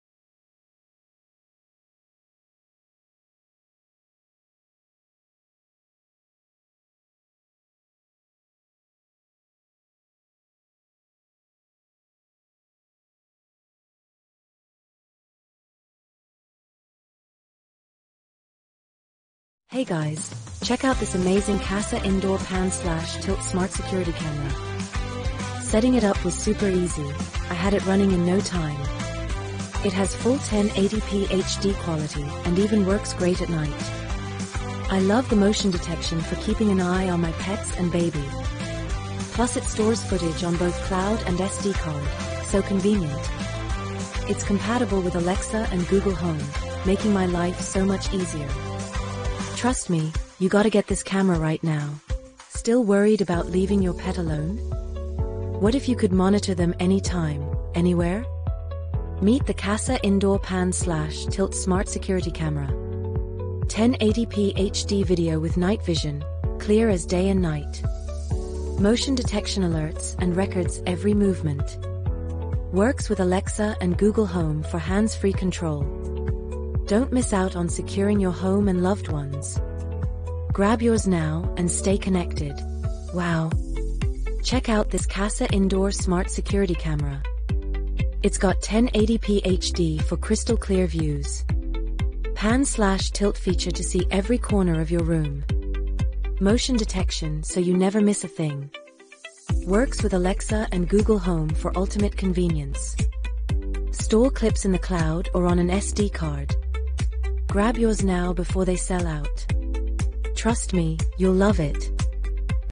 Security Camera, Keep Your Home Safe with Security Cameras
Video Description: Learn about the importance of indoor security cameras and how they can help protect your home from potential threats. Discover the latest features and technology available to keep your family and belongings safe.